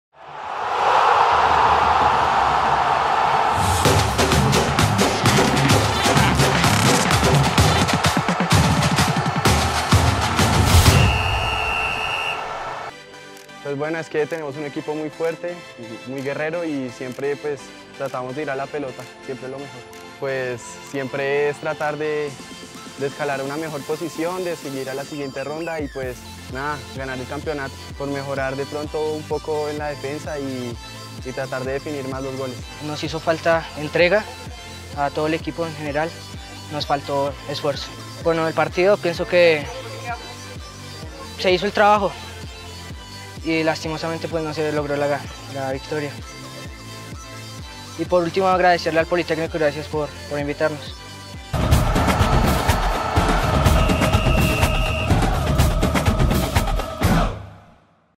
El Colombo Gales se impuso al Cooperativo Pablo VI
Una fecha más ha transcurrido en la Copa Poli. Esta vez se vieron cara a cara los equipos de los colegios Colombo Gales, de uniforme azul, y Cooperativo Pablo VI, de uniforme blanco con gris.